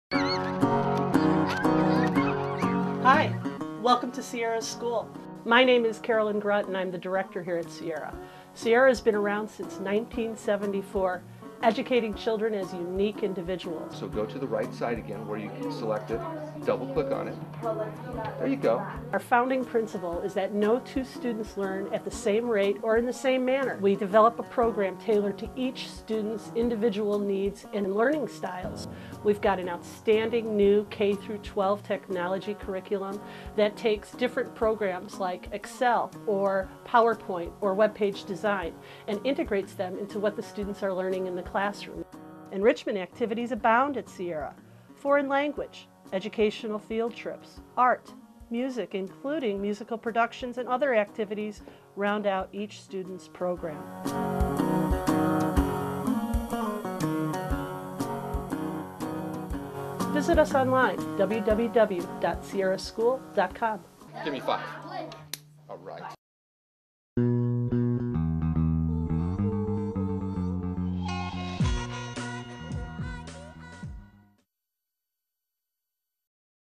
Santa Clara CA Private Instruction Sierra School
We all live and learn at our own rate. Sierra School in Santa Clara, California, understands that and has been offering small classes and individualized instruction since 1974. Offering kindergarten through high school classes, their curriculum includes science, math, athletics, web design, foreign language, creative writing and more for an enriching educational experience.